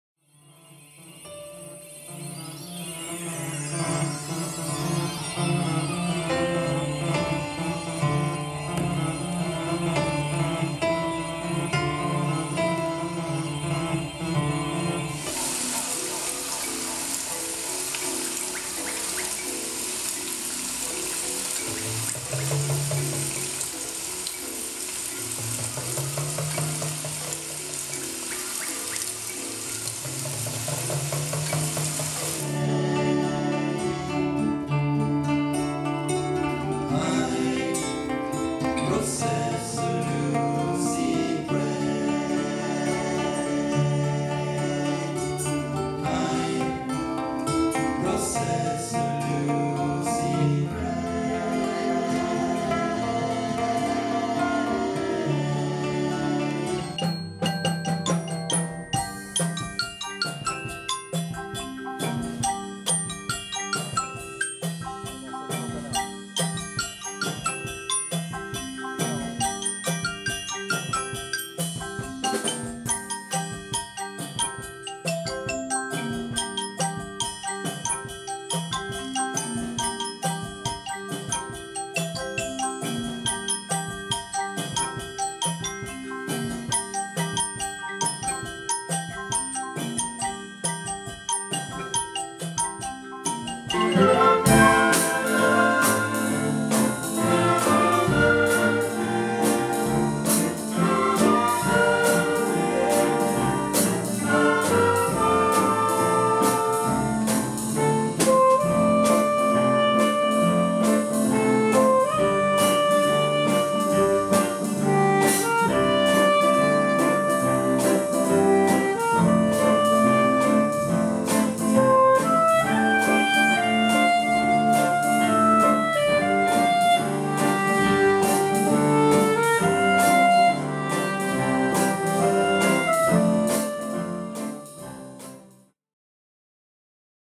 Châtellerault: première nocturne du patrimoine
Pour la première fois, à l'occasion des Journées du patrimoine, une nocturne était organisée samedi 17 septembre sur le site de la Manufacture: spectacle de cirque, musique avec la Compagnie laBase et le conservatoire, seul le "son et lumière" de Rustique et délic n'a pu avoir lieu en raison de la pluie.